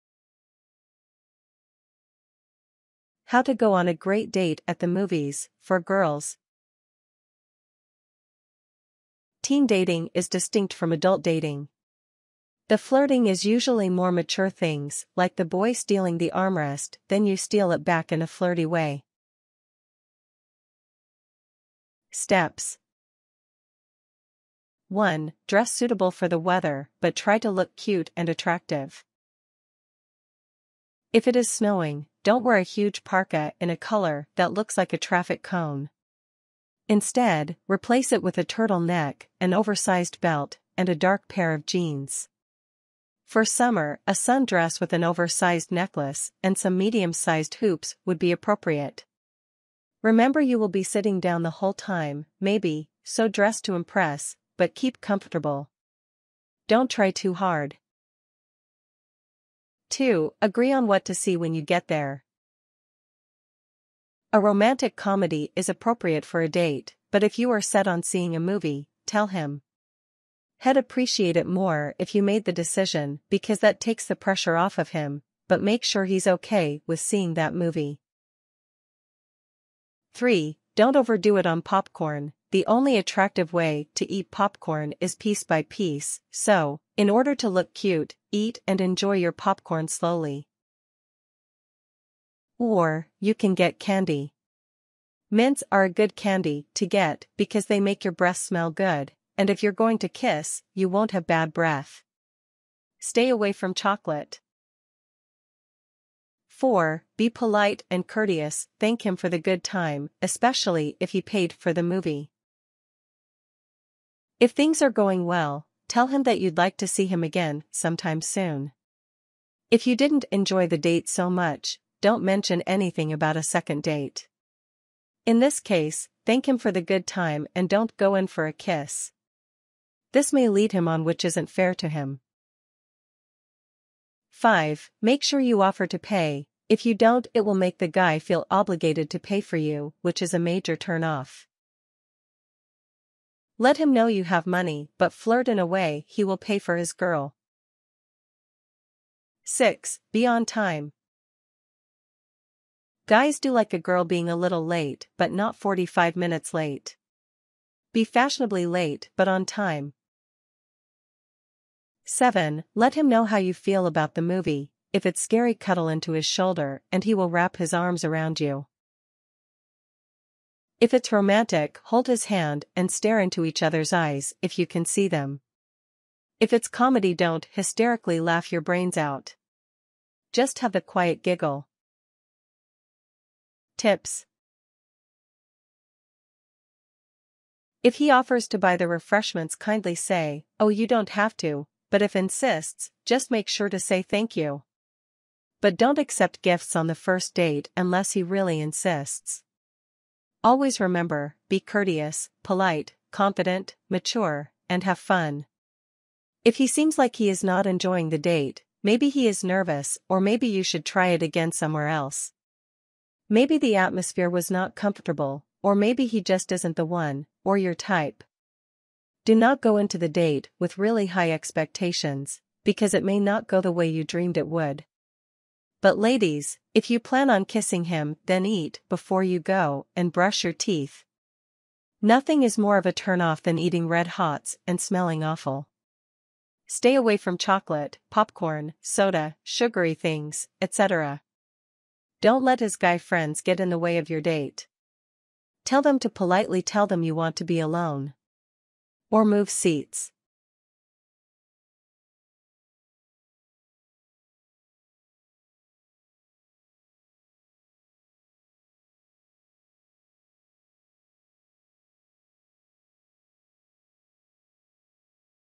Title: Dating 101: Stylish Outfits, Movie Choices, and Etiquette Tips for a Perfect Date Night!
In this video, we'll cover essential tips for a successful first movie date, focusing on "first date dos and don'ts," "movie date outfit ideas," and "etiquette tips." Step 1 emphasizes dressing appropriately for the weather while maintaining a cute and attractive look. Step 2 advises agreeing on the movie choice, suggesting that taking the lead can alleviate pressure. Step 3 highlights the importance of moderation when it comes to popcorn consumption and offers alternatives like candy, emphasizing mints for fresh breath. Step 4 underscores being polite and courteous, expressing gratitude for the date and signaling interest in future meetings if things go well. Step 5 recommends offering to pay to avoid making the guy feel obligated and advises being on time. Step 6 suggests conveying feelings about the movie through physical gestures, like cuddling during a scary scene or holding hands during a romantic one. The tips section includes advice on handling refreshment purchases, maintaining courtesy, confidence, and maturity, and not setting overly high expectations. Additionally, there's a reminder for ladies planning to kiss to eat beforehand and maintain fresh breath. Finally, a tip on managing interruptions from the guy's friends during the date is provided.